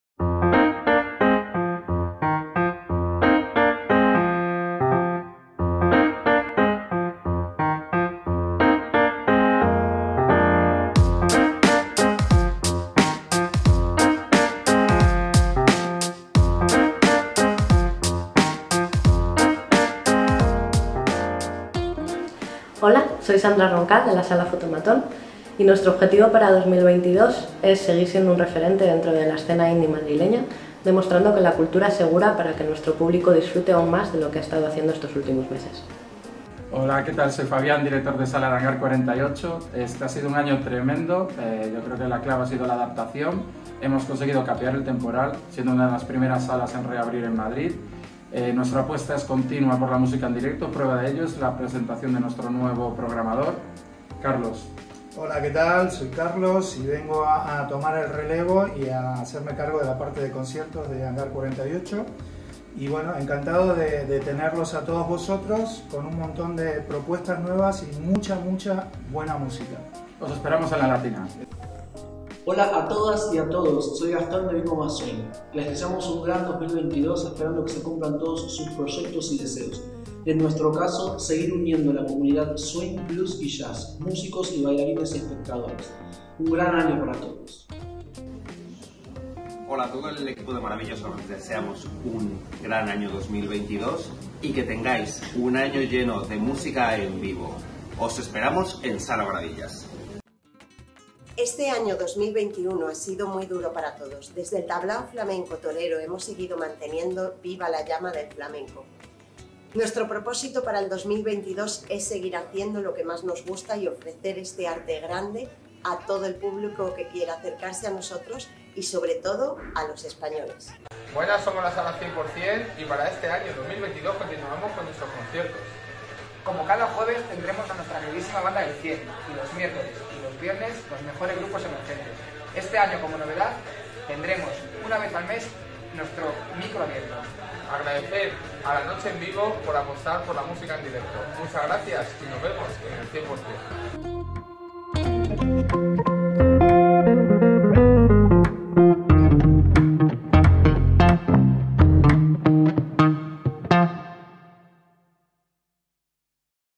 FELIZ 2022 LA NOCHE EN VIVO
Aunque venimos de dos años especialmente complicados, en La Noche En Vivo solo sabemos mirar hacia delante. Superarnos cada año y ofrecer la mejor experiencia cultural al público es el objetivo, tanto para nosotros, como para las salas de concierto de Madrid. Como prueba de ello, hemos realizado un pequeño vídeo para desearos un feliz 2022, lleno de música en vivo y experiencias memorables en las salas.

Agradecer su participación en el video a Sandra Roncal de la sala Fotomatón, Fabián Novoa y Carlos Lezcano de Hangar 48, Gastón Fernández de Big Mama, y a los equipos Maravillas,  Torero-Essential Flamenco y la sala Cien por cien.